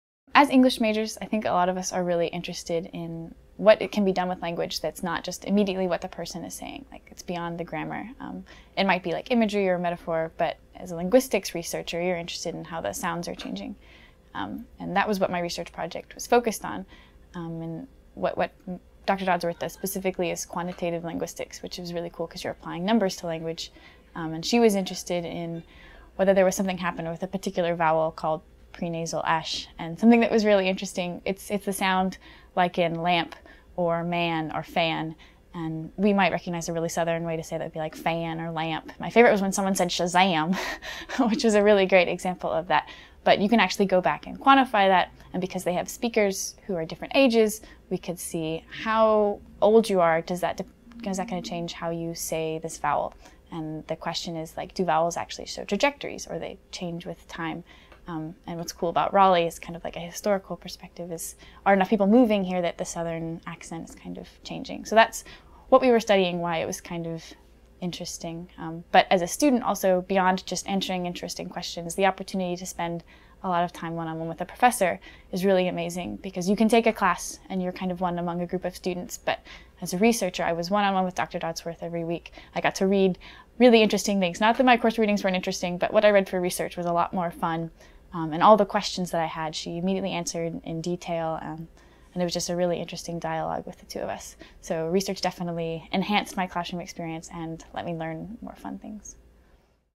NC State Undergraduate Research: Heidi Klumpe
Heidi Klumpe, a Park Scholar at NC State, discusses what it was like to do research on sound change as an undergraduate while taking an Introduction to Linguistics course through NC State's English department.